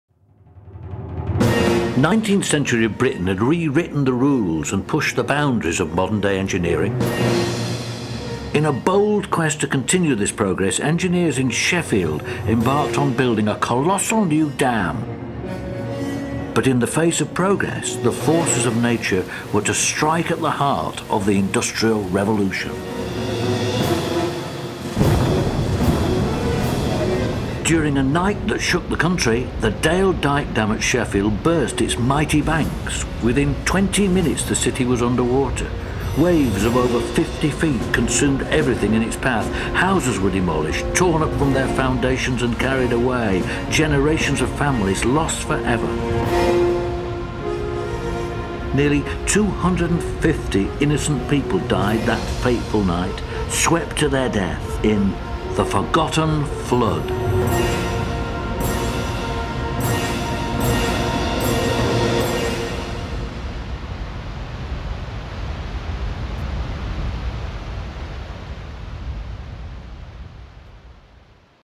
The Forgotten Flood: Great Sheffield Flood 1864
It's 150 years since Sheffield was engulfed by water after the Dale Dyke Dam burst its banks. This is the story of the Great Sheffield Flood that killed over 250 innocent people and wrecked a modern Victorian city.